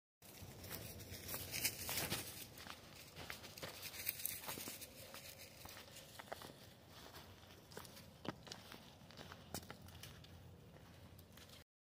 I could be Jason From Friday The 13th. Don't I have the walk?
I tried to put myself in Jason Voorhees shoes. I think I did pretty good.